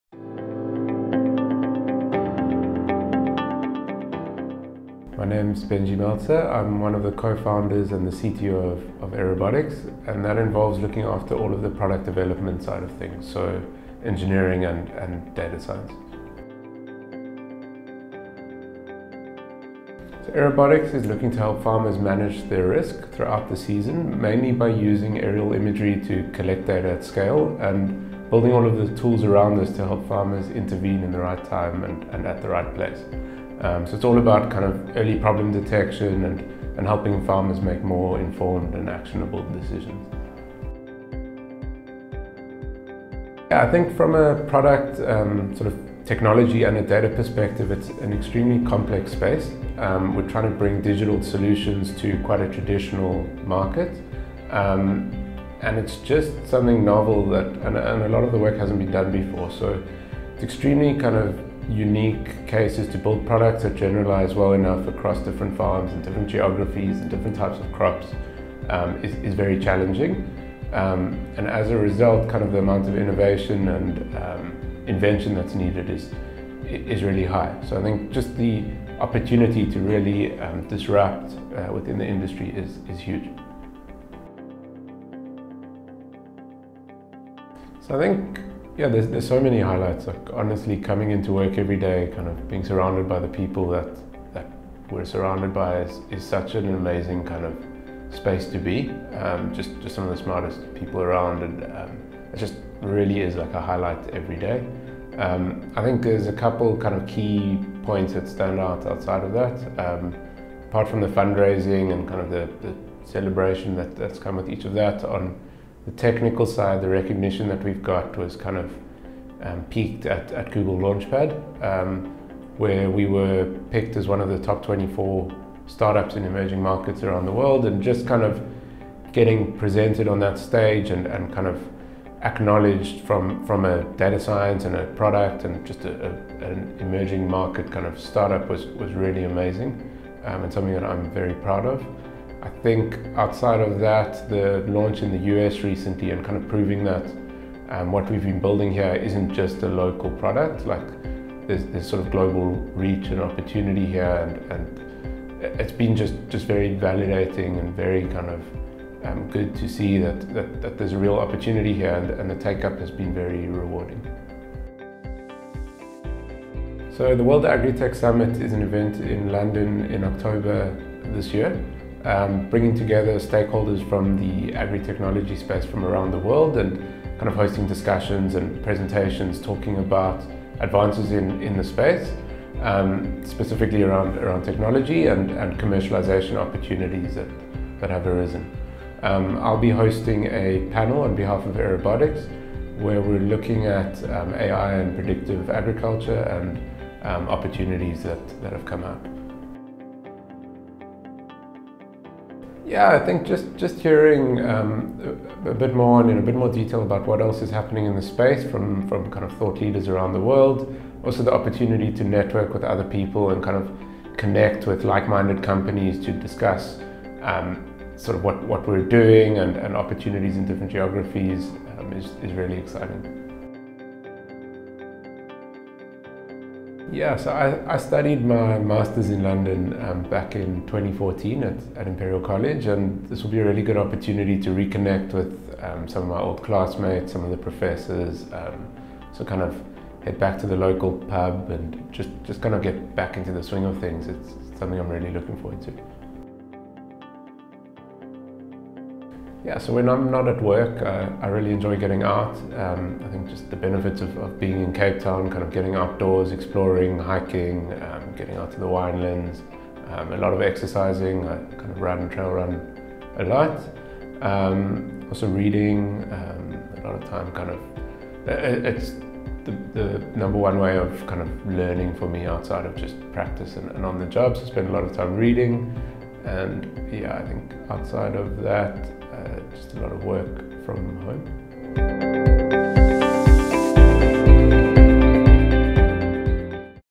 Another Day at Aerobotics: Meet Benji Meltzer (Co-Founder & CTO)
Ahead of hosting a roundtable discussion at the World Agri-Tech Innovation Summit in London, meet our CTO & Co-Founder, Benji Meltzer!